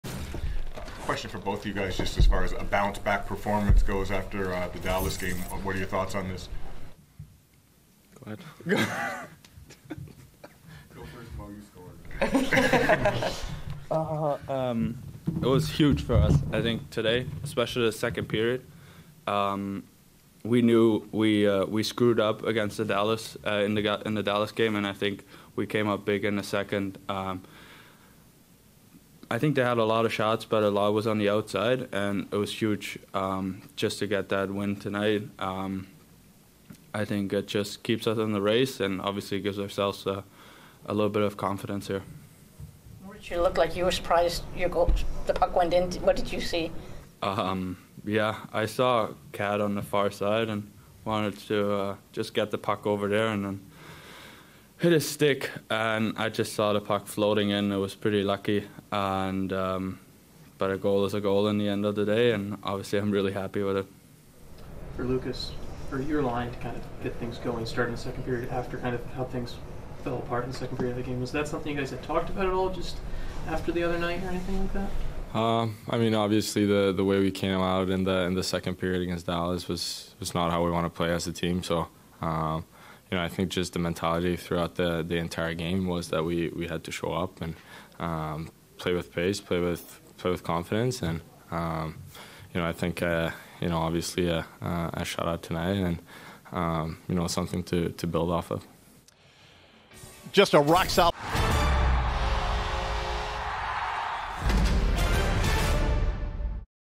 Red Wings LIVE 1.25.24: Lucas Raymond Moritz Seider
After the Red Wings defeat the Flyers 3-0, we hear from Lucas Raymond and Moritz Seider in their postgame reactions.